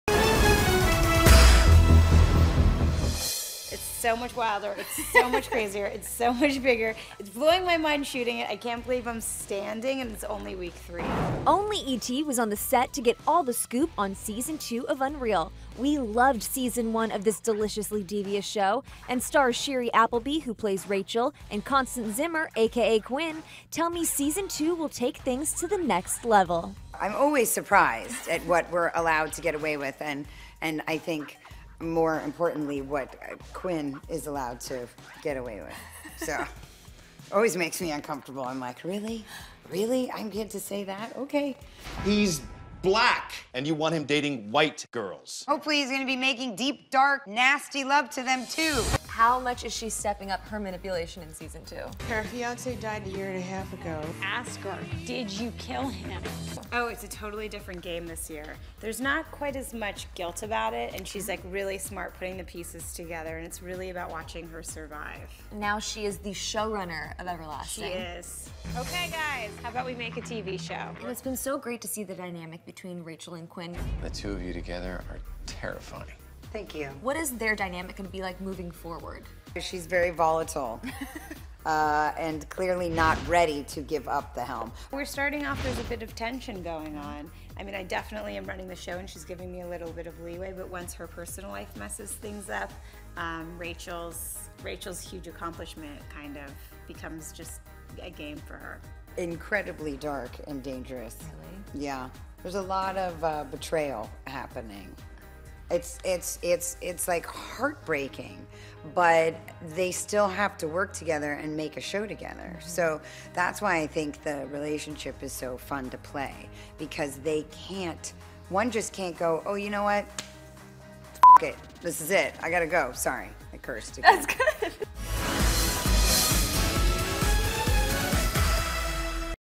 EXCLUSIVE: Shiri Appleby and Constance Zimmer on 'Wilder, Crazier' Season 2 of 'Unreal'
The 25-year-old actor was taken into custody by the LAPD for battery on Sunday.